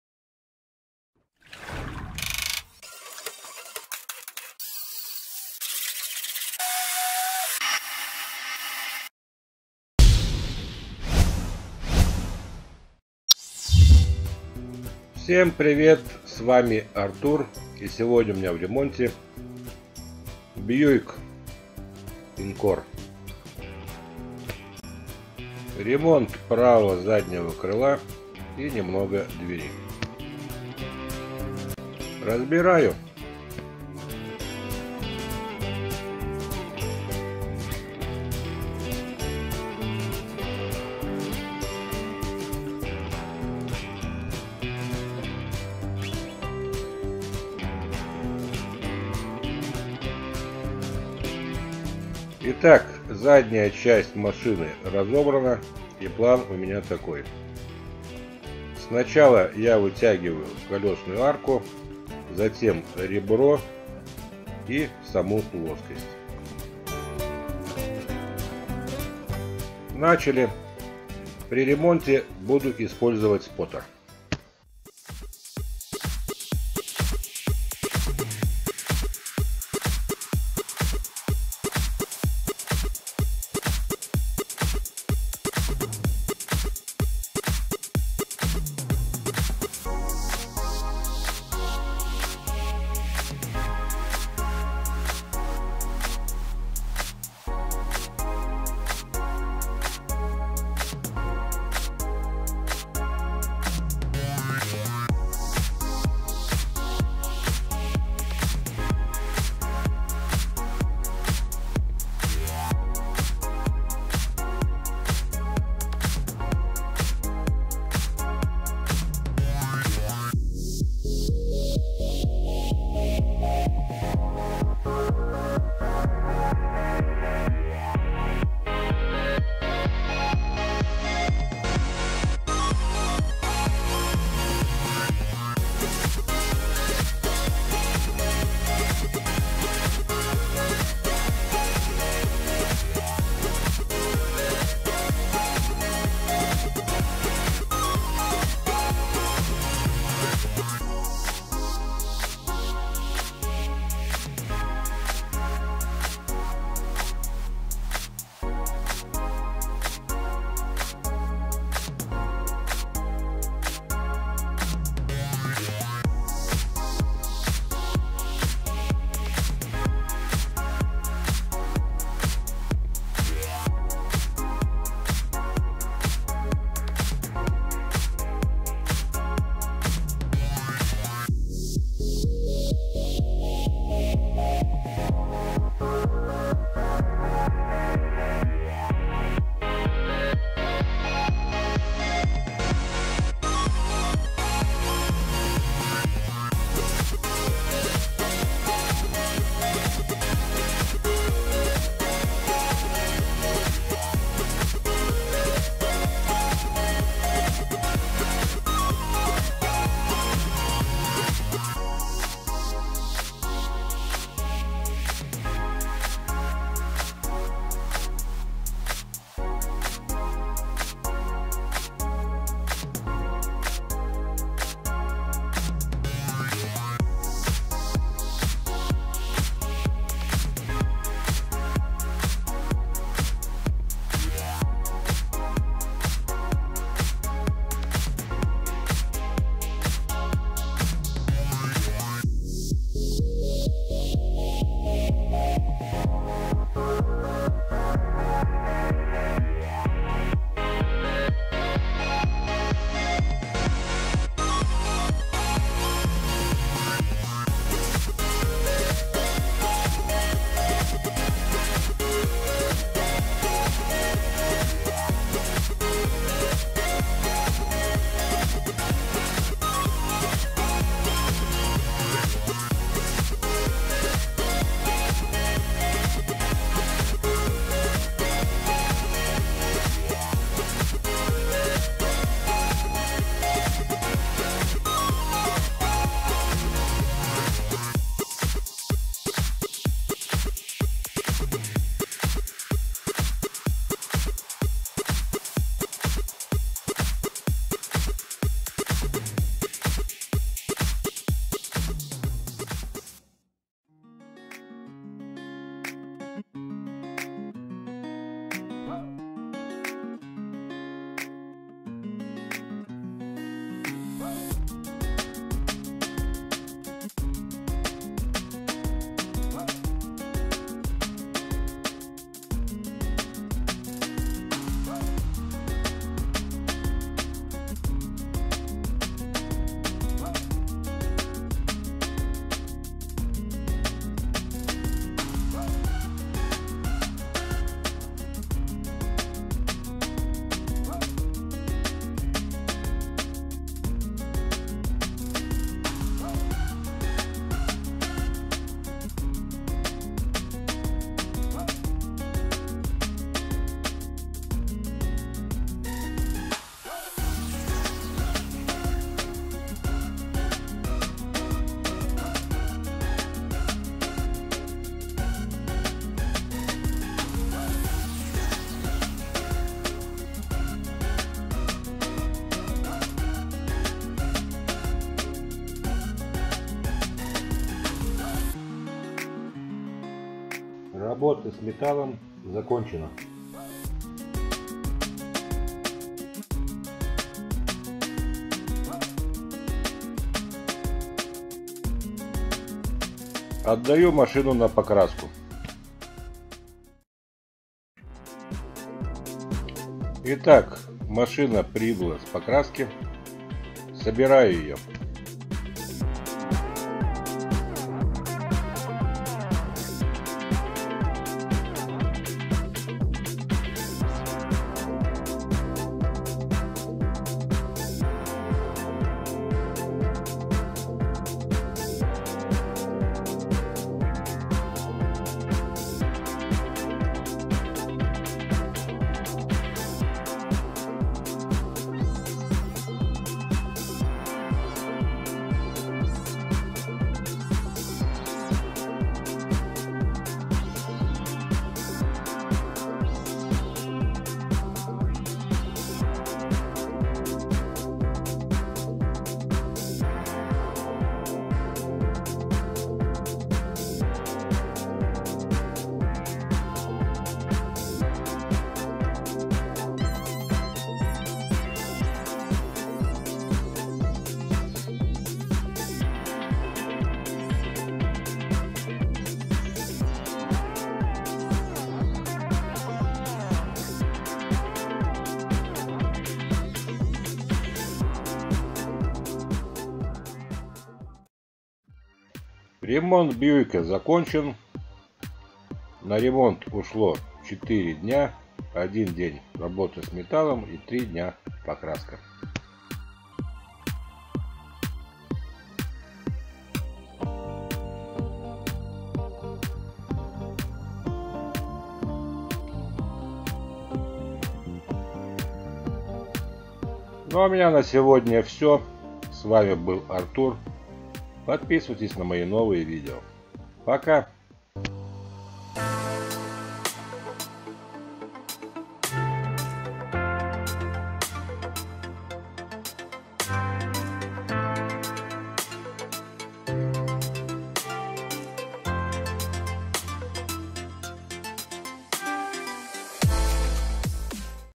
Buick. The quarter panel repair. Ремонт заднего крыла.
Body repair. Work with a spotter.
Ремонт кузова. Работа со споттером.